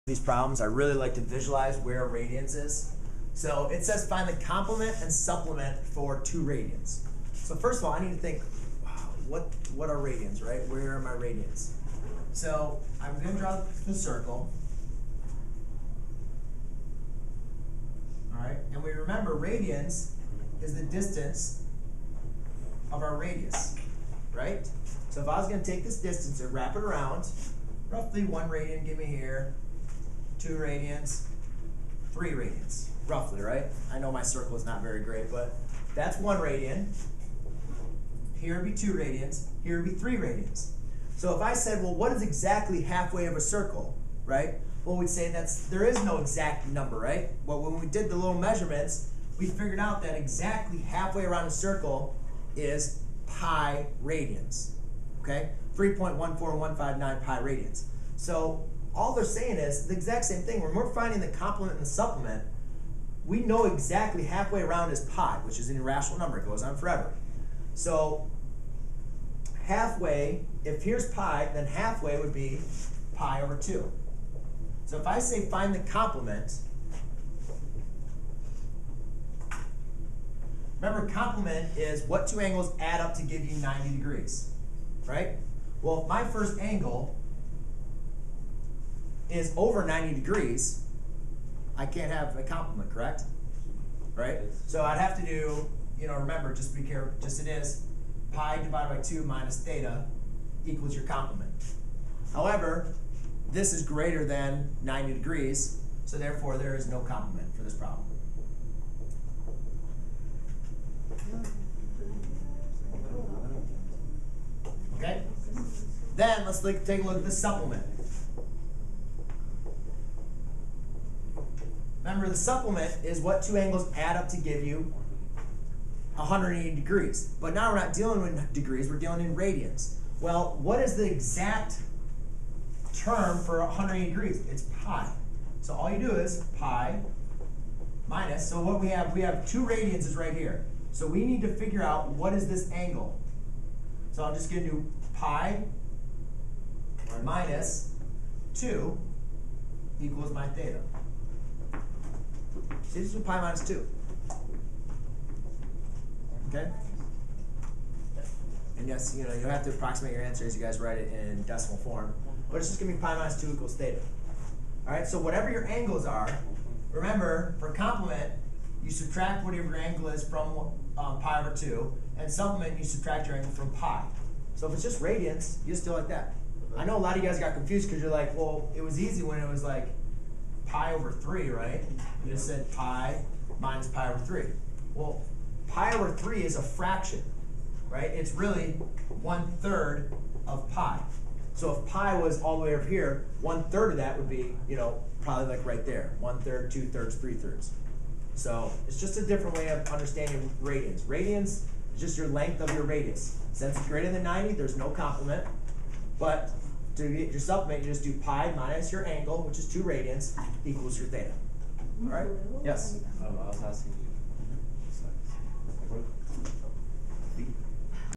Finding the complement and supplement for 2 radians
👉 Learn how to find the complement and the supplement of a given angle. Two angles are said to be complementary if they add up to 90 degrees (pi/2 in radians) and are said to be supplementary if they add up to 180 degrees (pi in radians). To find the complement of a given angle in radians we subtract the given angle from pi/2 and to find the supplement of a given angle in radians we subtract the given angle from pi.